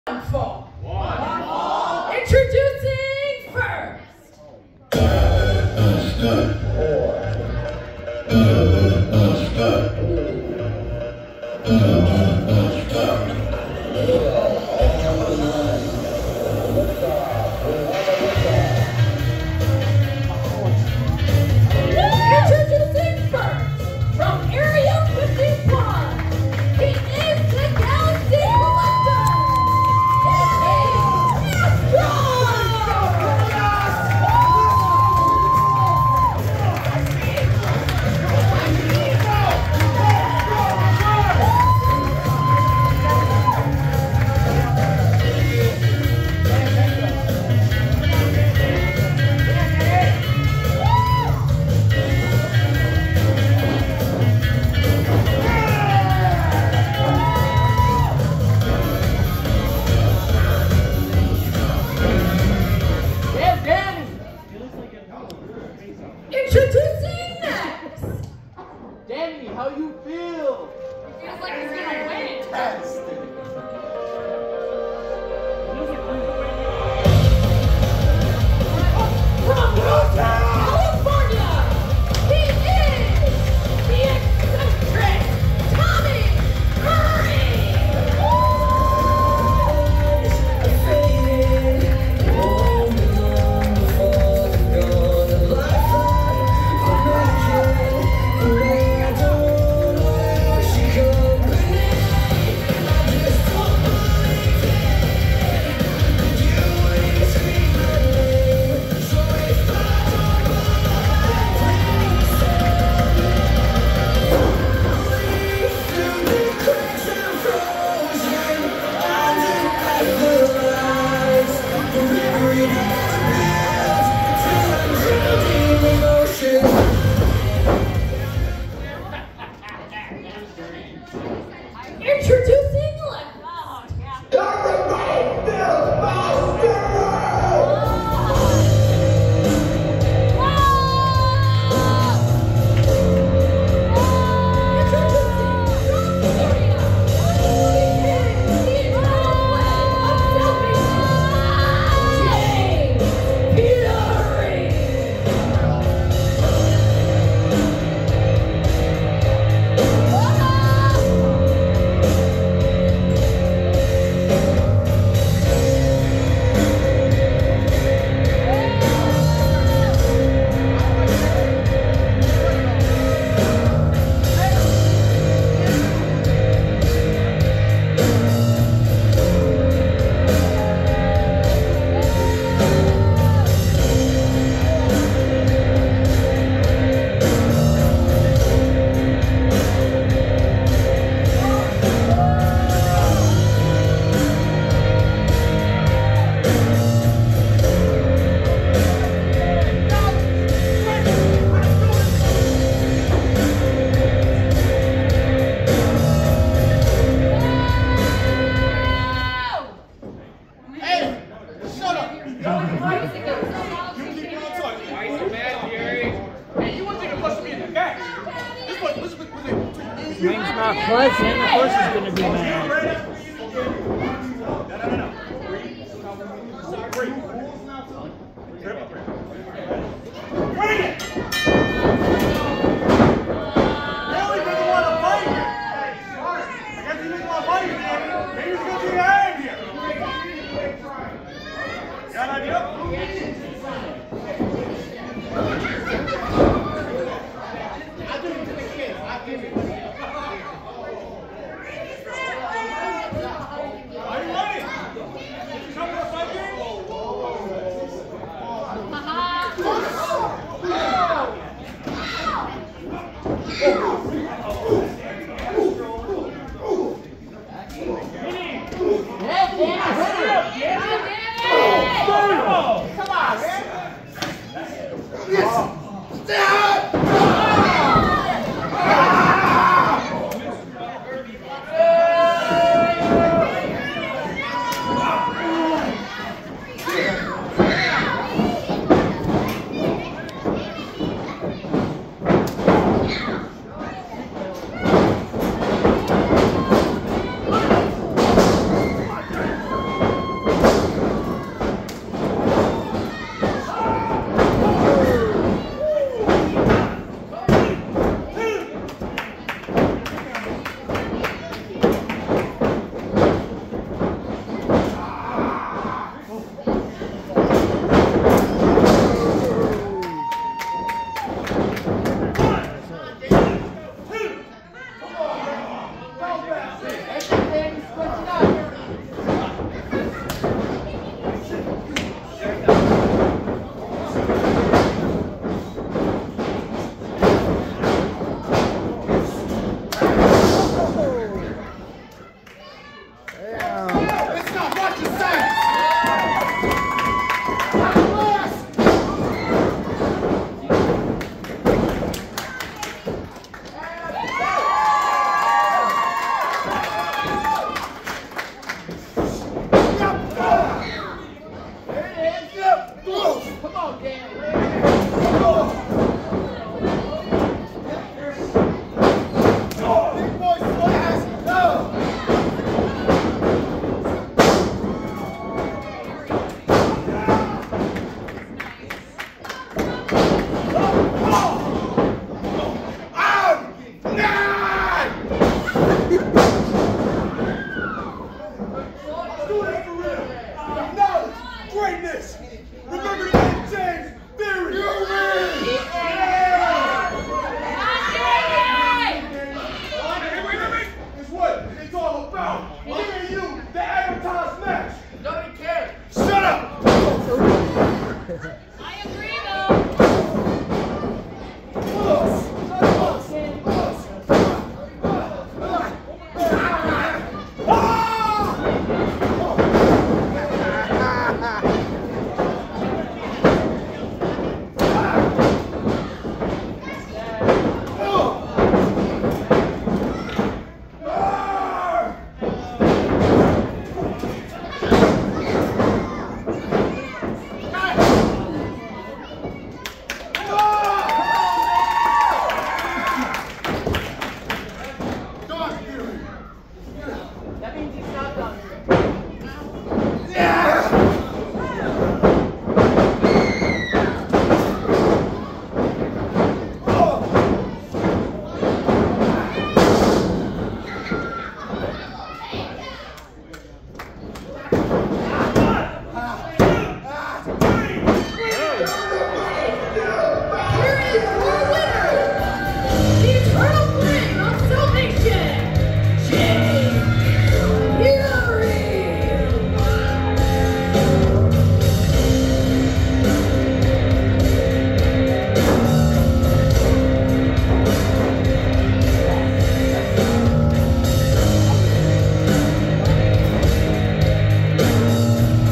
Triple Threat Match Danny Astro vs Tommy Rinks vs James Fury
KAOS Wrestling Organization Insurrection 2 9/7/24